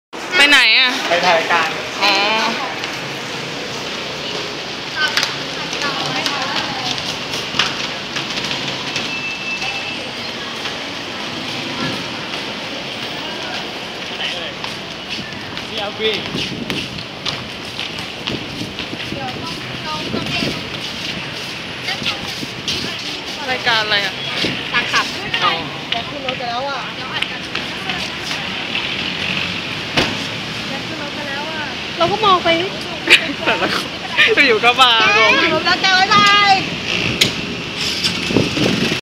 แกงส้ม Kangsom ออกจากตึก@GMM-9/8/56
แกงส้มออกจากตึกไปถ่ายรายการสตาร์คลับ
วันศุกร์ที่ 9 สิงหาคม 2556 เวลา 13.30 น.